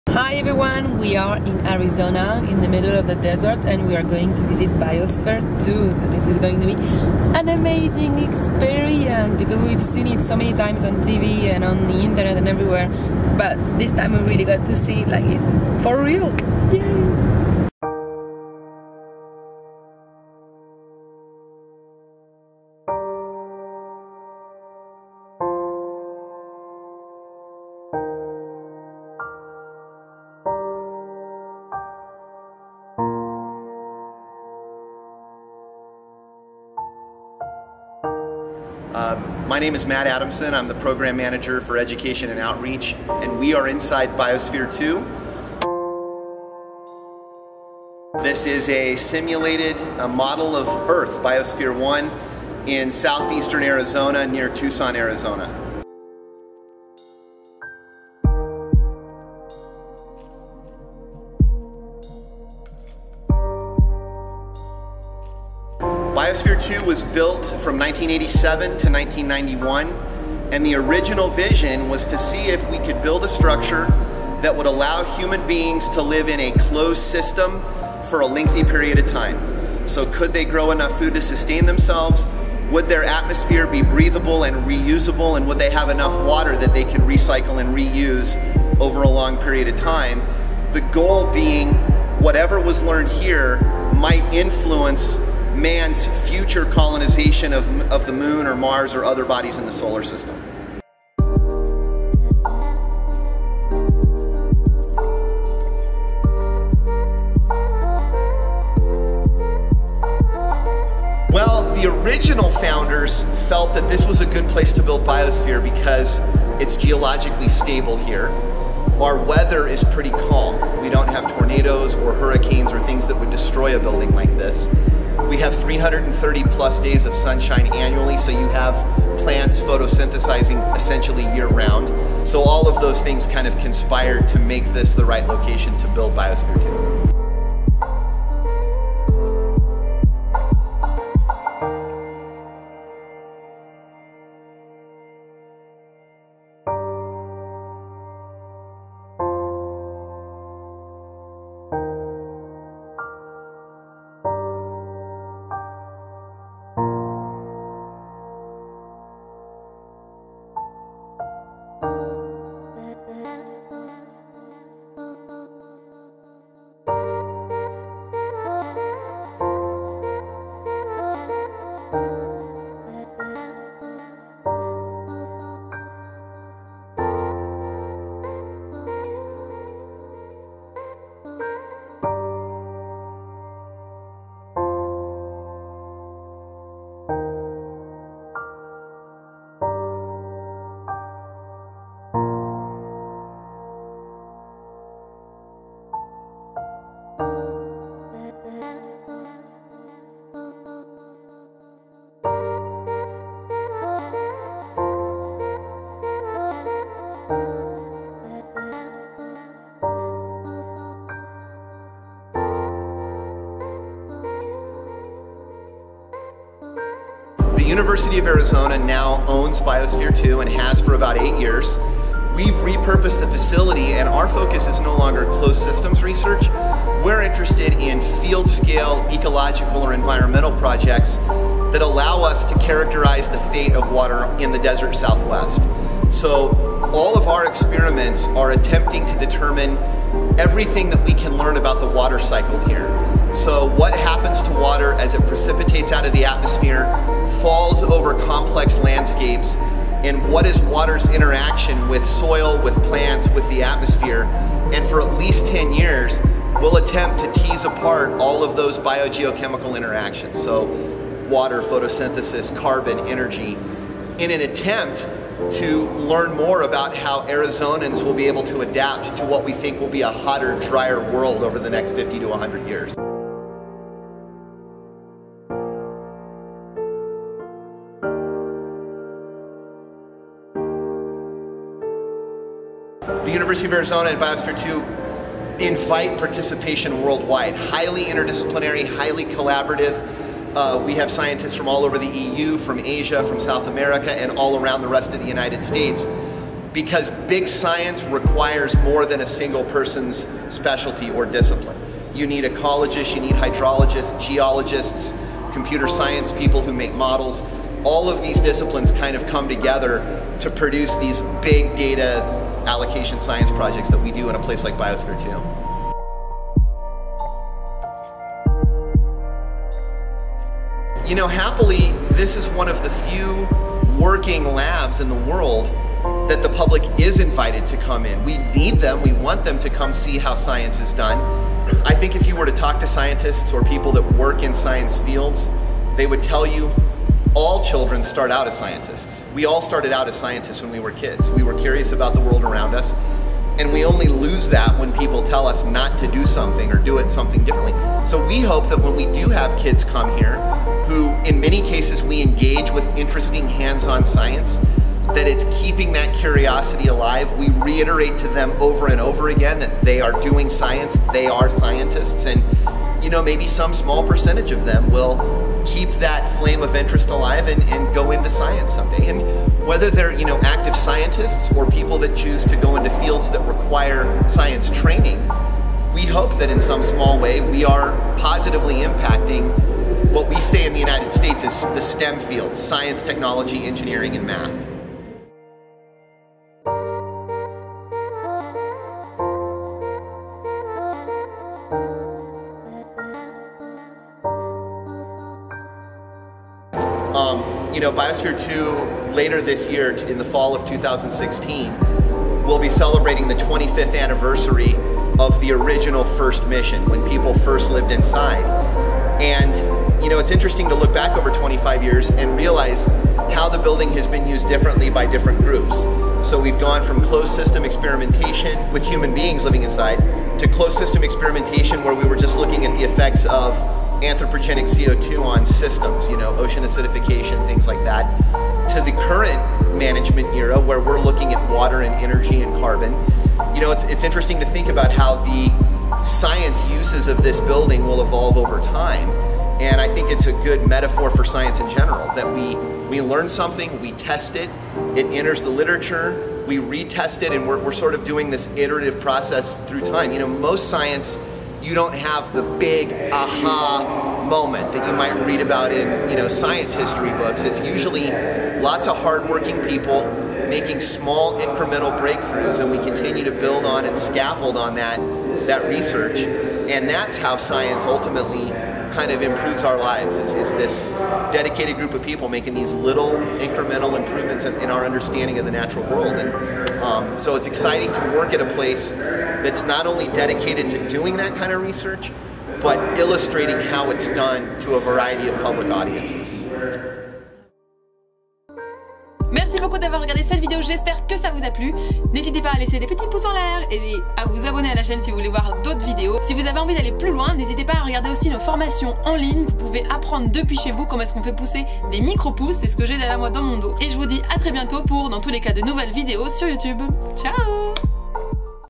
Biosphere 2 - Hydroponics in Arizona - HWT #37
Hello everyone ! Biosphere 2 is a place full of wonderfull things and amazing stories. I hope you will enjoy it as much as I did !

► ABOUT US:
Marion & Nicolas - Les Sourciers

⚡️!! NOUVEAUX: FORMATION EN LIGNE !!⚡️
Comment démarrer son projet de culture de micropousses MICROGREENS FOR PROS